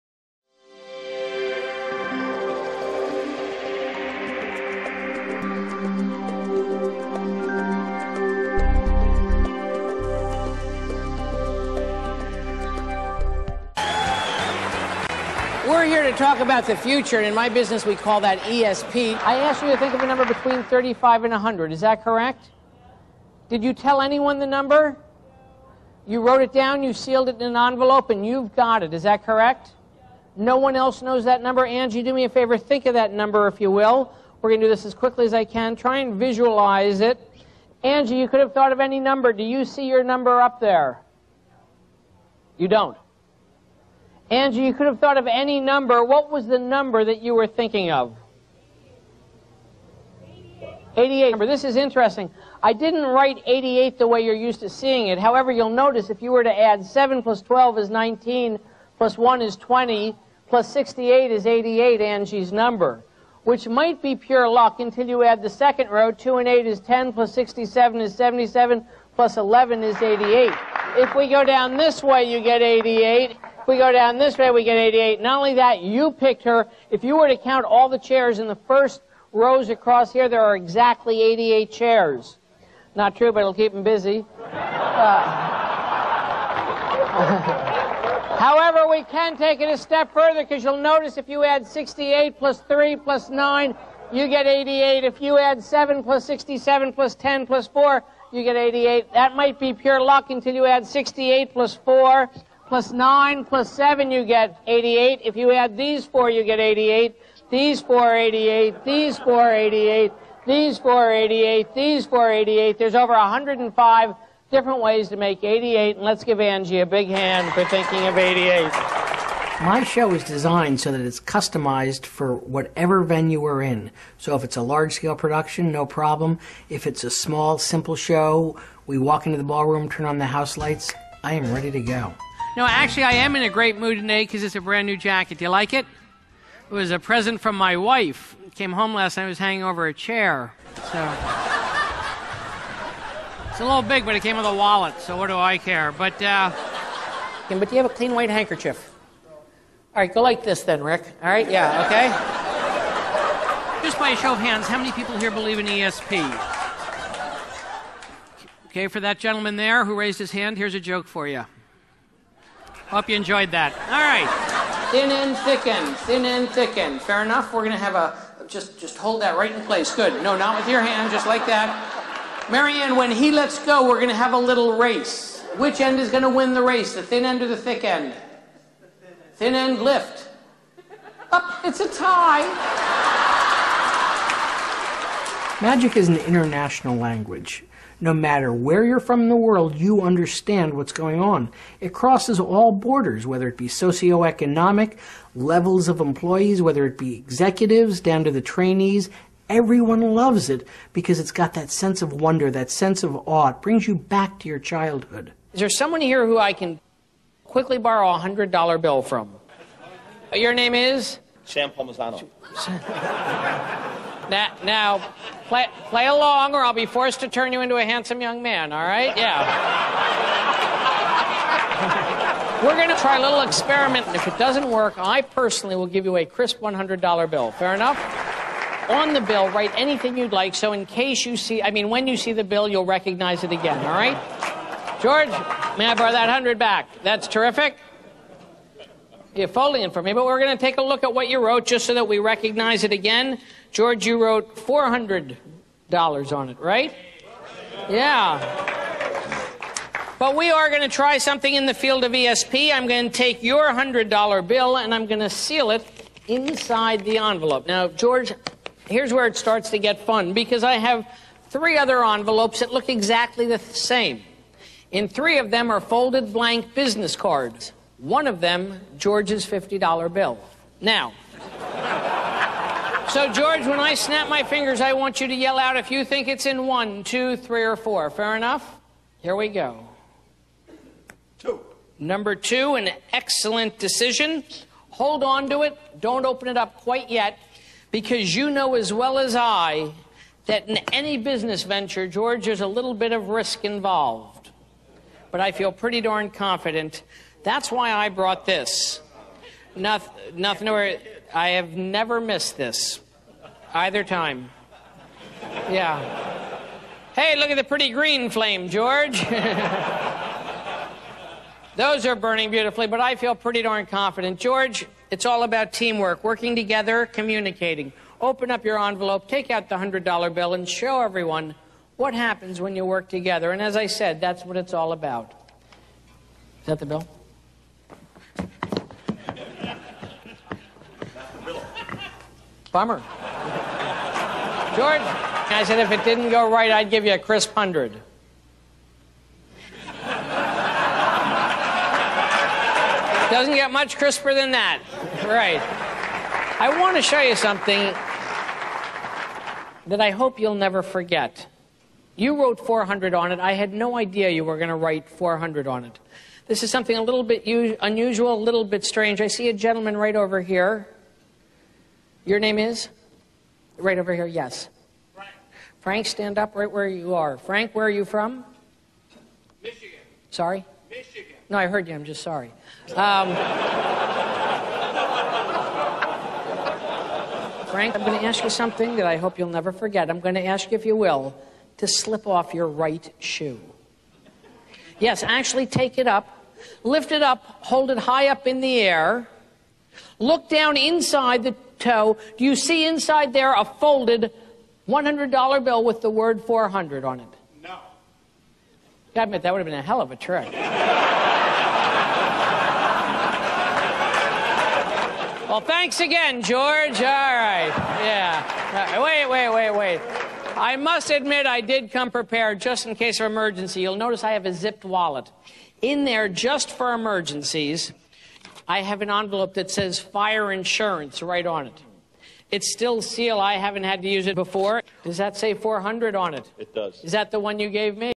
Bill Herz Corporate Magician
BILL HERZ  is regarded as one of the most entertaining corporate magicians in the world.  Except for taking a little time out to earn a bachelor's degree from Amherst College and a master's degree from Cornell University, Bill has spent the past 20 years performing at corporate events. Bill's unique mix of comedy and magic has been applauded at corporate functions throughout the U.S., Canada, and Europe.  He is the author of Secrets of the Astonishing Executive, published by Avon Books.   Whether performing during a meeting, entertaining clients after dinner or astonishing guests at a cocktail party with his sleight of hand, Bill always integrates audience participation, humor, and his unforgettable wizardry to ensure a special event that's truly special.